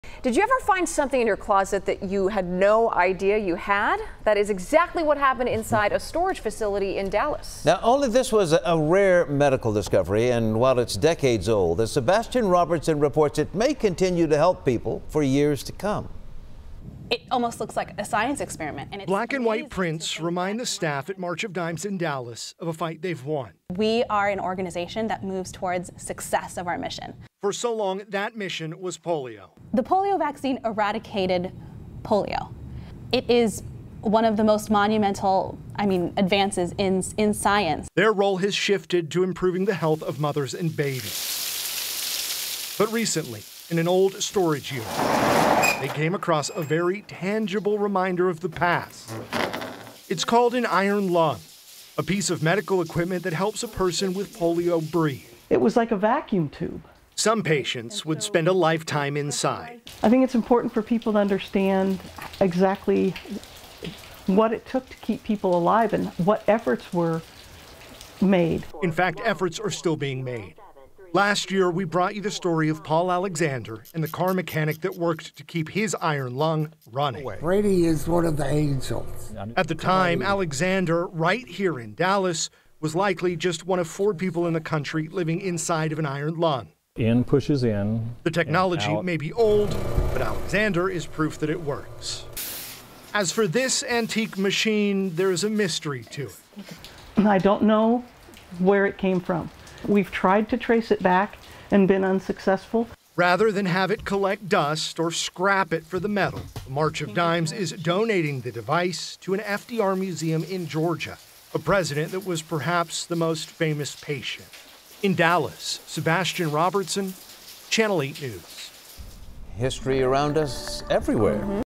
March of Dimes staff finds iron lung in storage unit
For decades the organization's focus was on Polio, an infections disease that can cause life long paralysis. In the 1950s it was described as one of the most feared diseases in the United States.